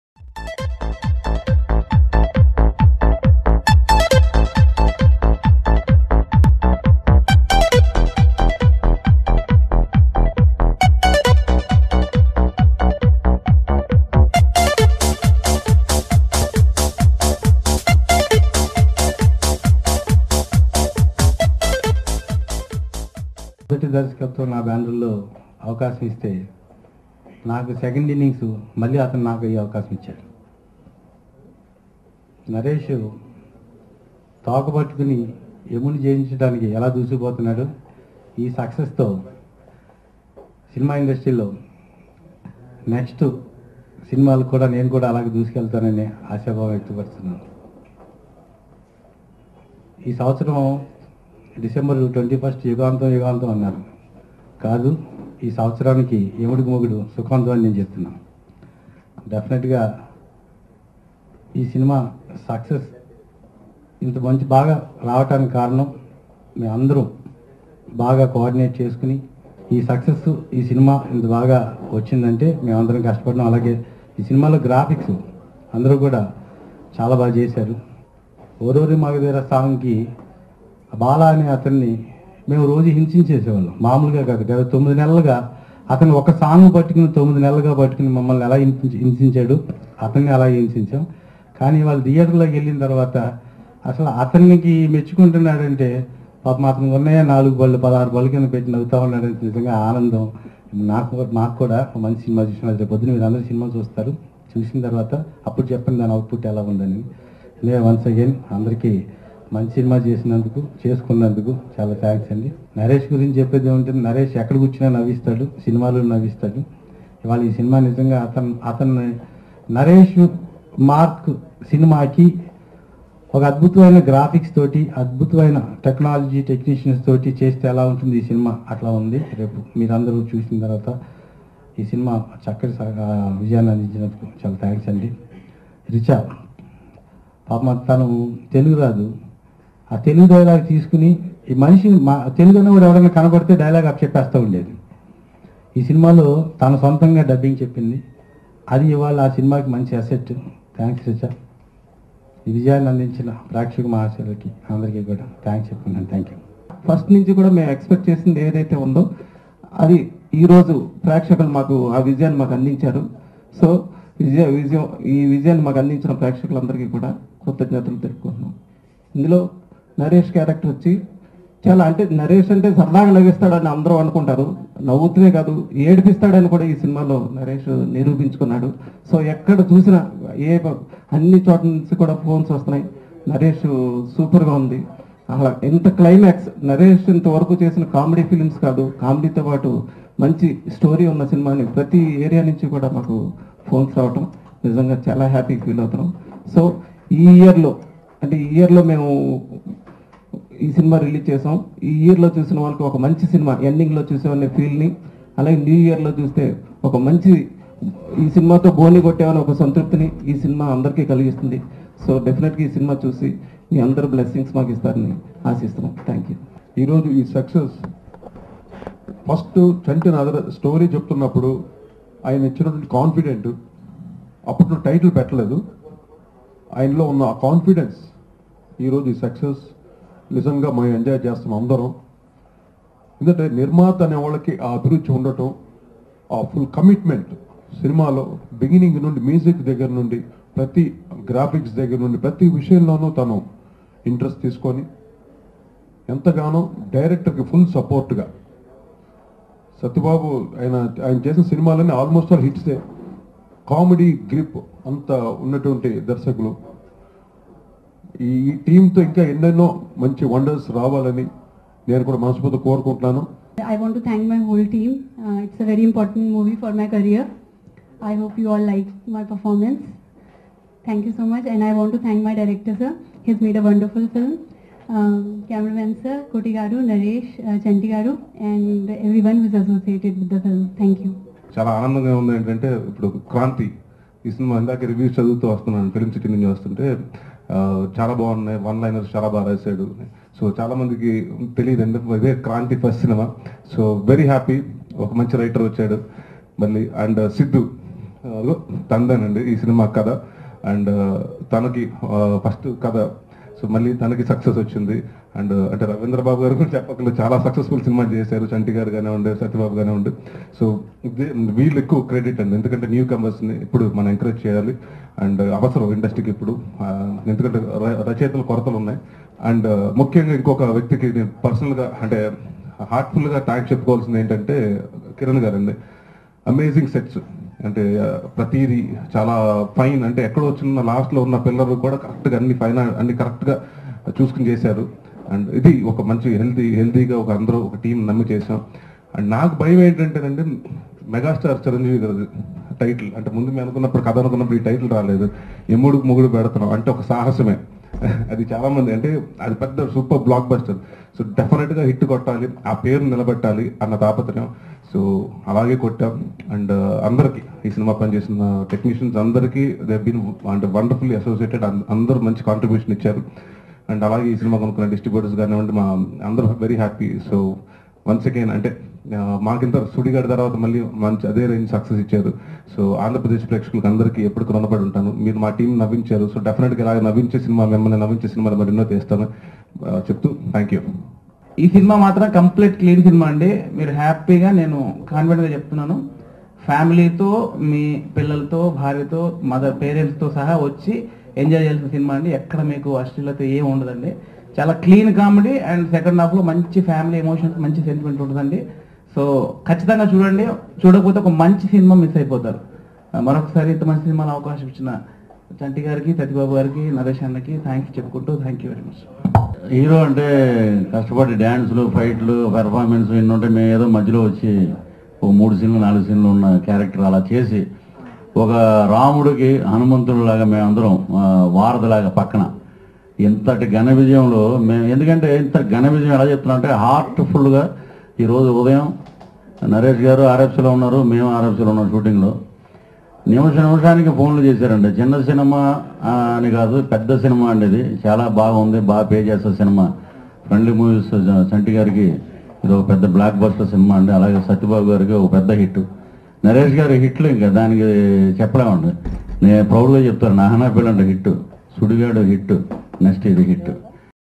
Yamudiki Mogudu Movie Success Celebrations
Allari Naresh - Richa Panai - Ramya Kirshna - Yamudiki Mogudu Movie Success Celebrations and Yamudiki Mogudu Unit Success Meet